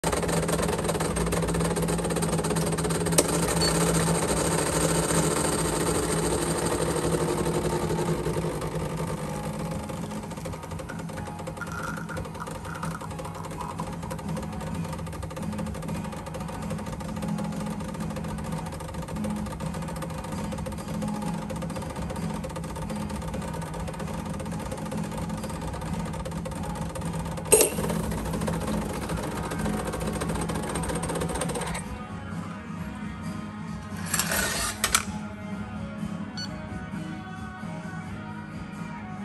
Espresso extraction using the Decent DE1 with an E61 profile
This shot was using the E61 profile on the Decent DE1 XL (v1.4). Beans were a medium roast, around 4 weeks from the roast date.

- E61 Profile
- No pre-infusion, straight up to 9bar pressure.
- 19g in, 36g out.
- Around ~23 seconds